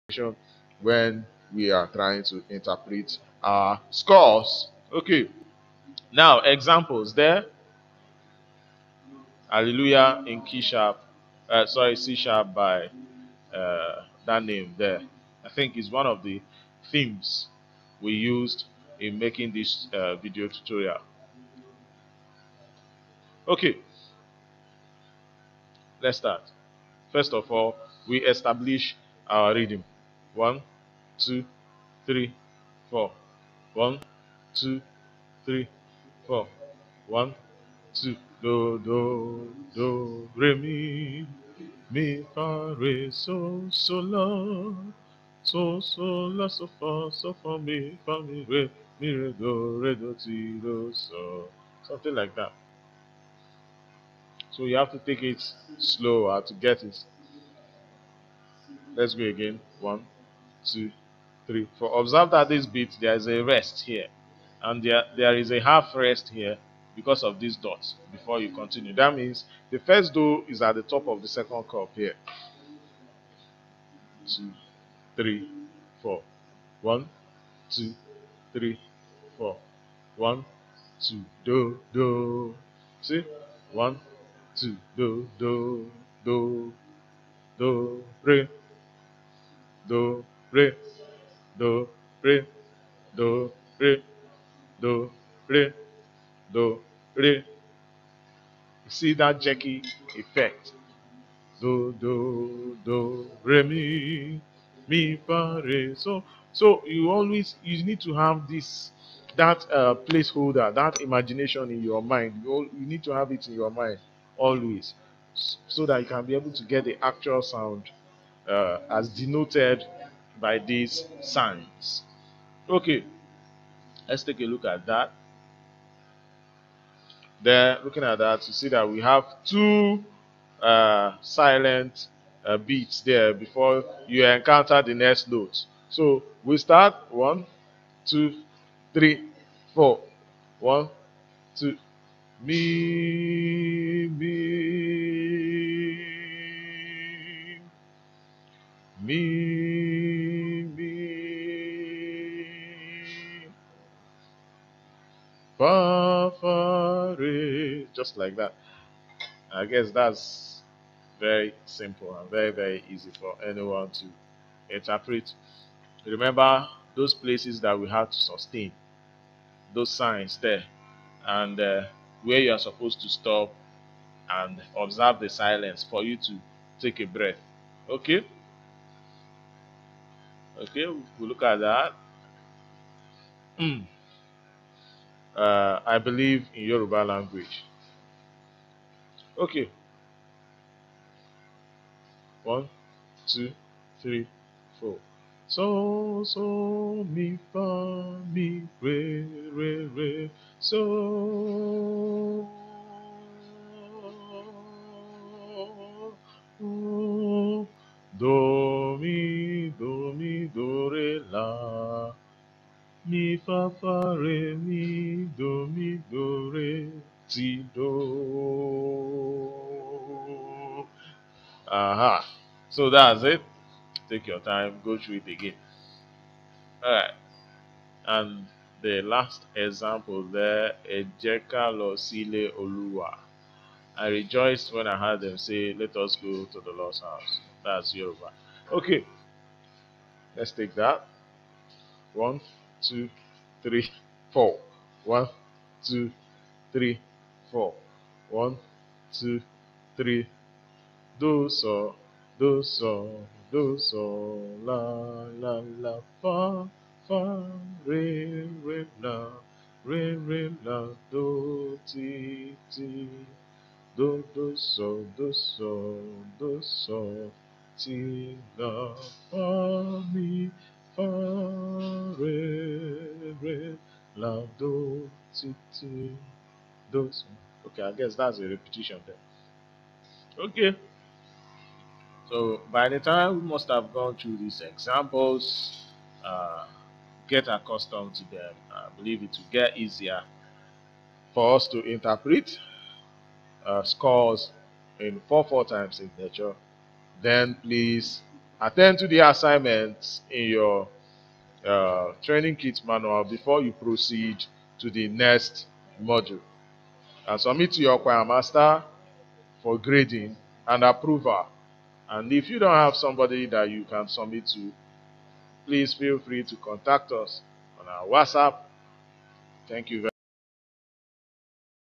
SolFa Interpretation Tutorial Module 6Part 5 Interpreting 44 time signature
Learn how to interpret SOLFA notation from beginner to advance level.